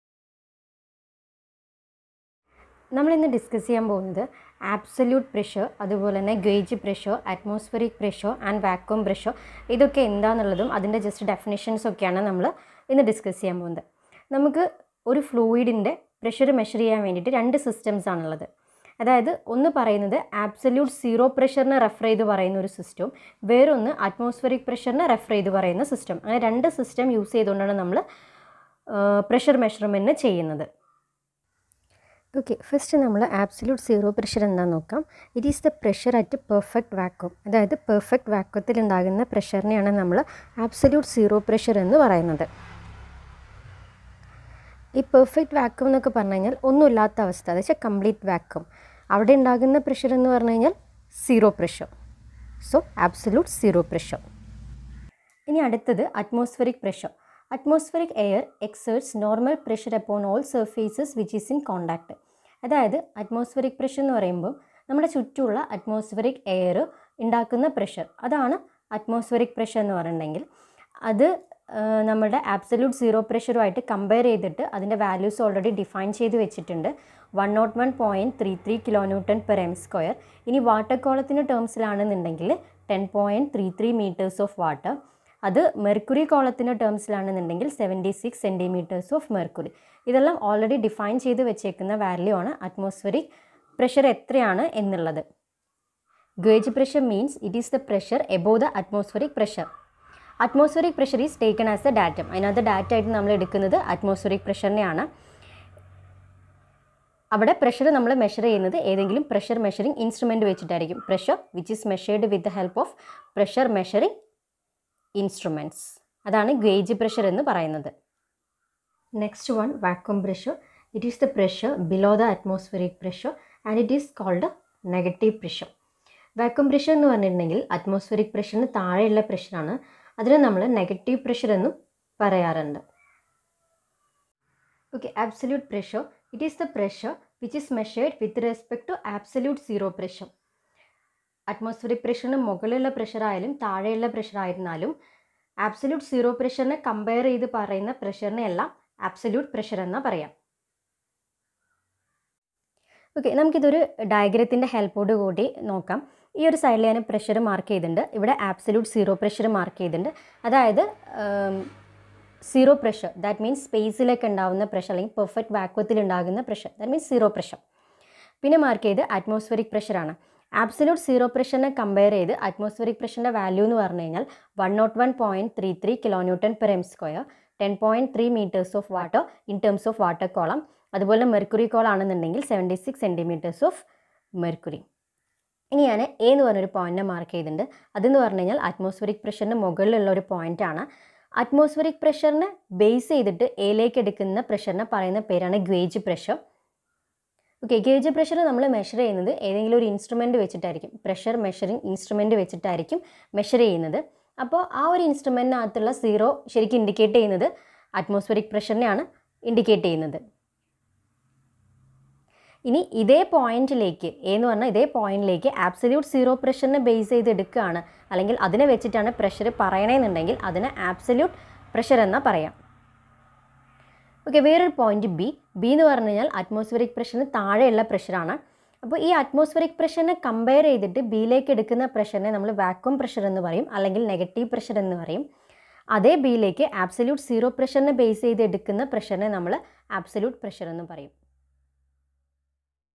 DEFINITIONS OF ABSOLUTE PRESSURE, VACUUM PRESSURE, GUAGE PRESSURE, ATMOSPHERIC PRESSURE
Definitions of Absolute pressure, Vacuum pressure, Guage pressure, Atmospheric pressure, Absolute zero pressure in Malayalam